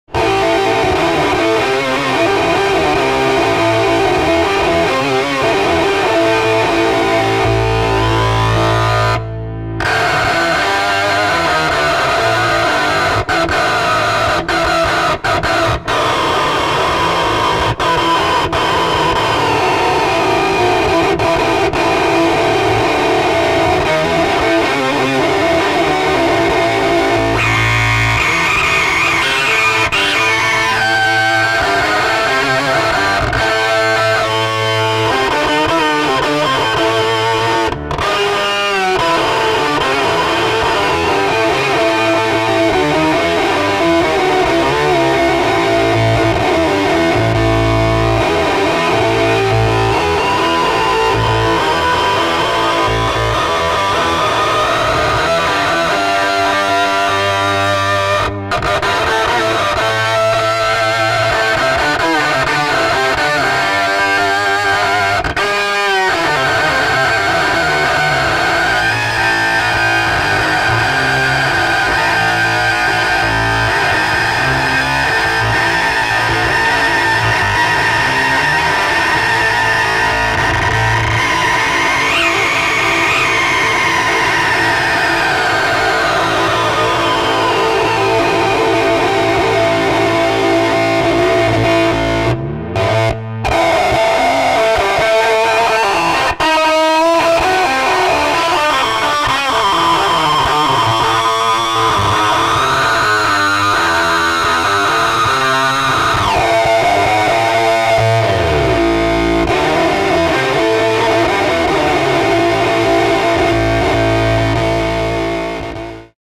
Nicolas Collins' Custom Fuzz Dicer and Digitech Ricochet Demo
A crude demo using the Ricochet as an octave down pitch shifter.